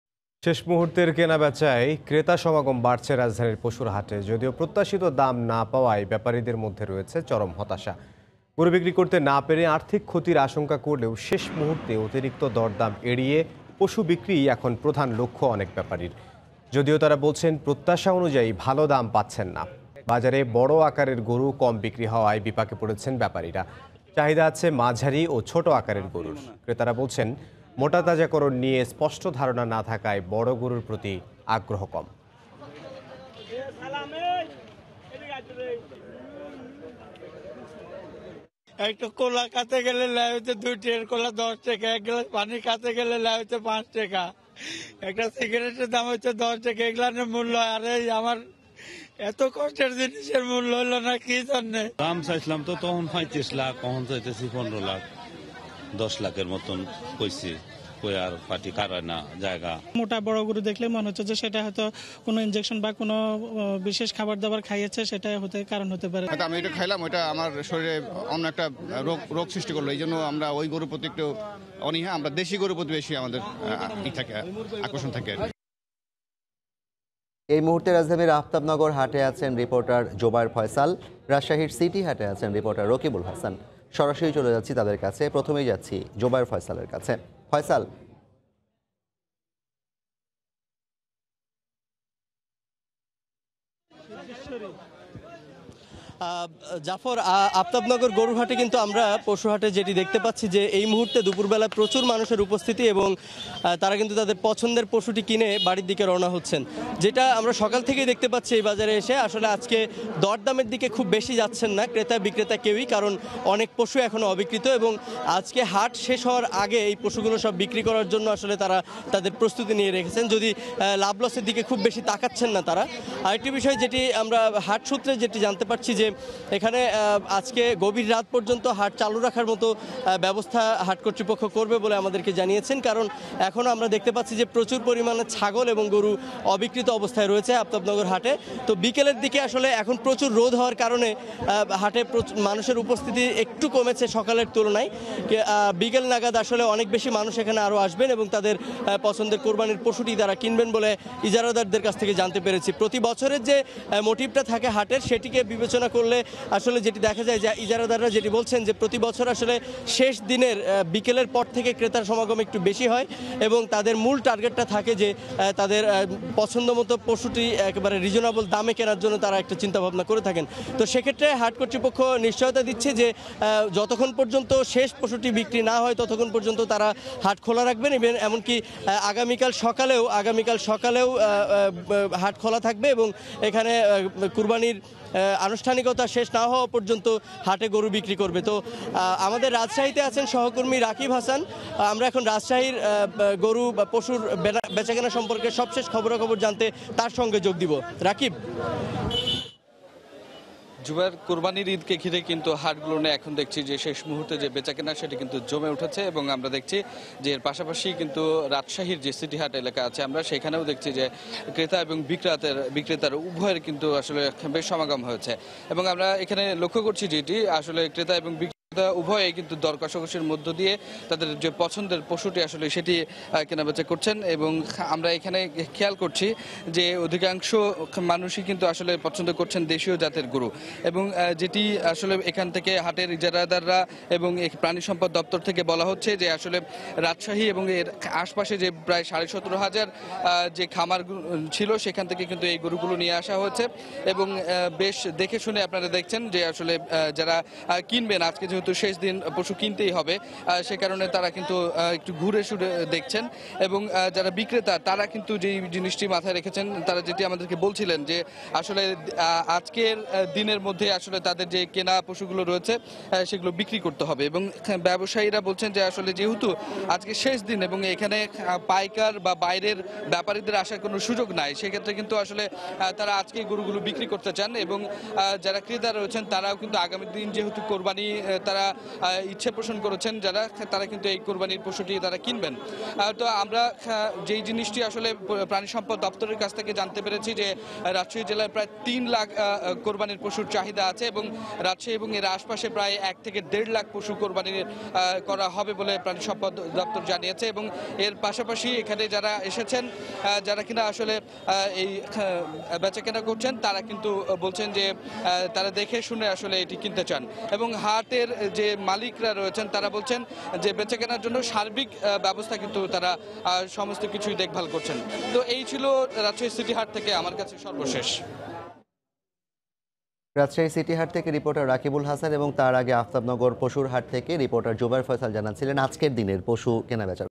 গরু বিক্রি করতে না পেরে কান্না করছেন ব্যাপারিরা! | Cow Hat | Somoy TV
শেষ মুহূর্তের কেনাবেচায় ক্রেতা সমাগম বাড়ছে রাজধানীর পশুর হাটে। যদিও প্রত্যাশিত দাম না পাওয়ায় বেপারিদের মধ্যে রয়েছে চরম হতাশা।